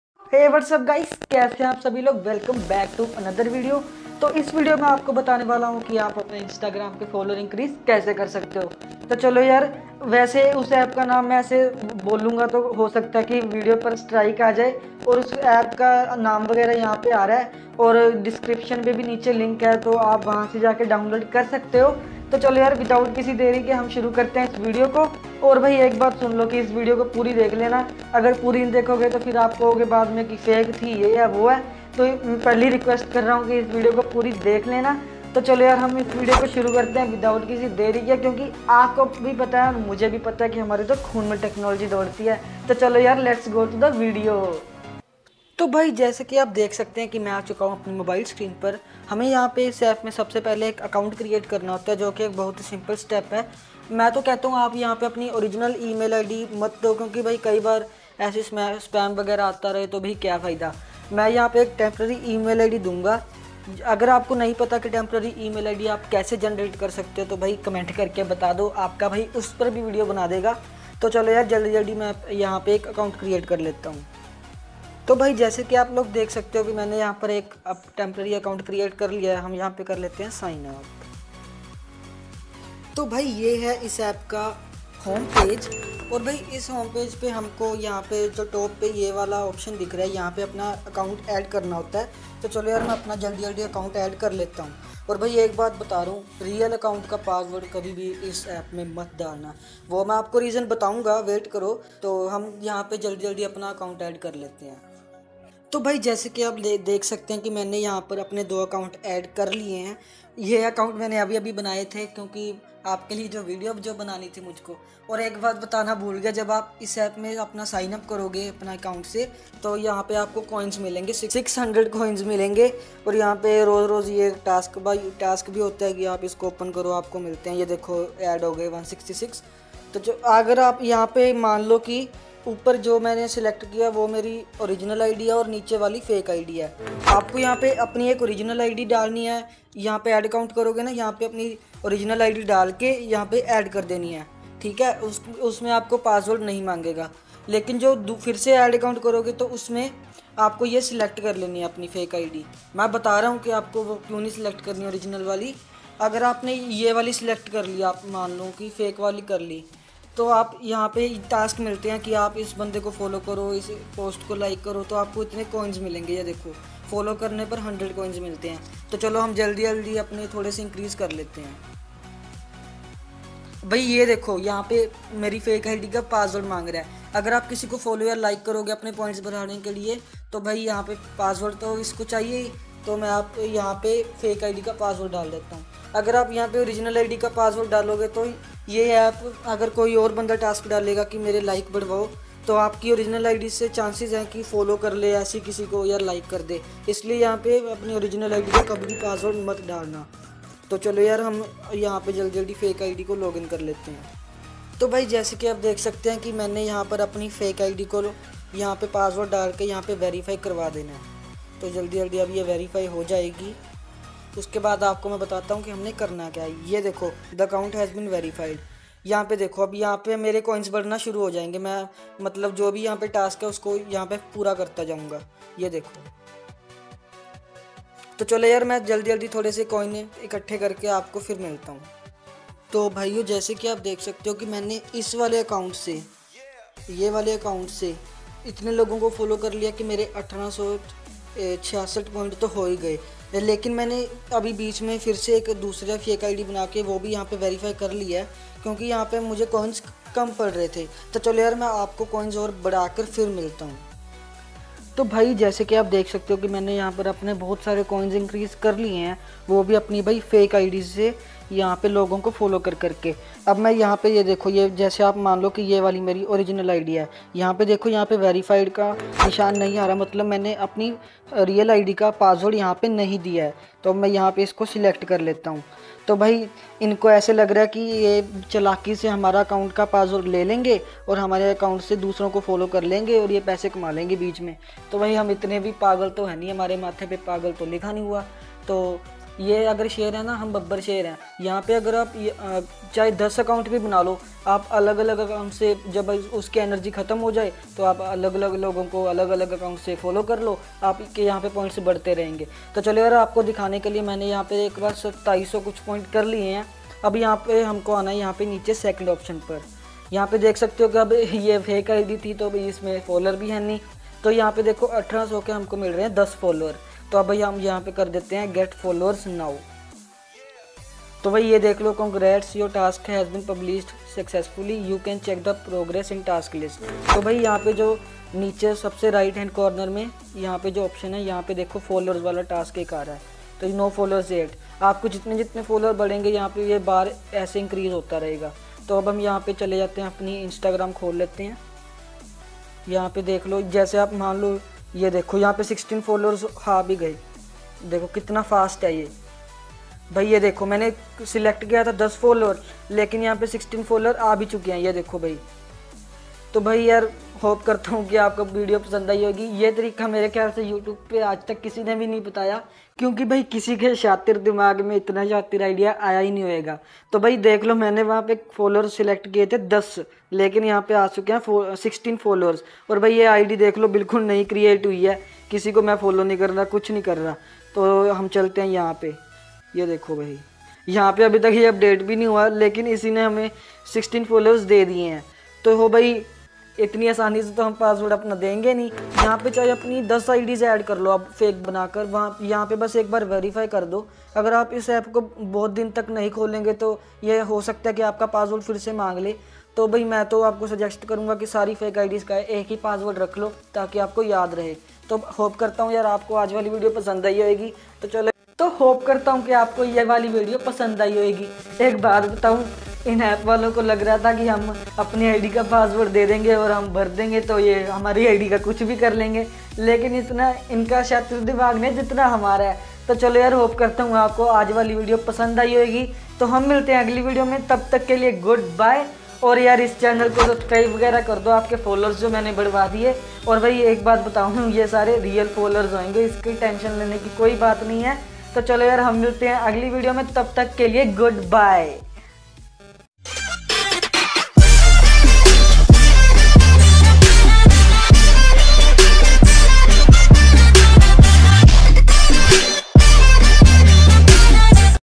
How To Increase Followers on Instagram *Latest Trick* | 2021 Hindi | Techy Blood
Hello Guys in this video I am going to show that How you can increase your instagram followers in just a minute with a awesome trick in Hindi.

So Good Bye Guys See u in the next video.

Ye mat bhulna ki HAMAARE TO KHOON MEI HI TECHNOLOGY DODTI HAI.

Queries Solved In This Video are